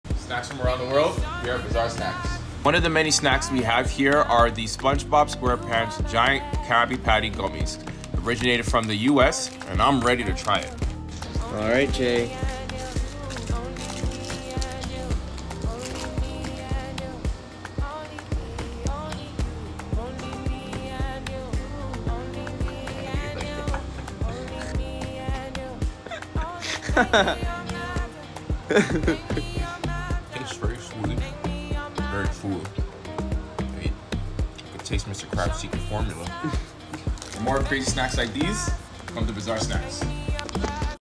TASTE TESTING GUMMY KRABBY PATTY (MUST WATCH!?) 🤤😳
Trying Gummy KRABBY Patties at our Candy Store